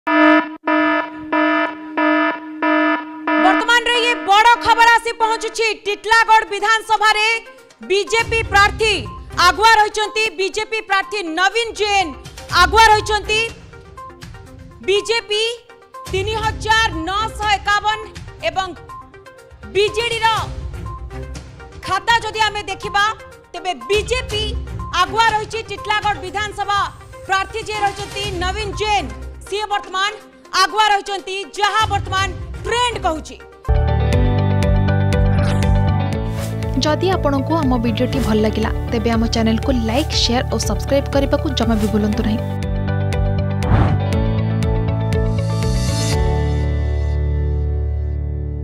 ଟିଟିଲାଗଡ ବିଧାନସଭା : ବିଜେପି ପ୍ରାର୍ଥୀ ଆଗୁଆ || Election 2024 Result
ଟିଟିଲାଗଡ ବିଧାନସଭା : ବିଜେପି ପ୍ରାର୍ଥୀ ଆଗୁଆ । ବିଜେପି ପ୍ରାର୍ଥୀ ନବୀନ ଜୈନ ଆଗୁଆ ।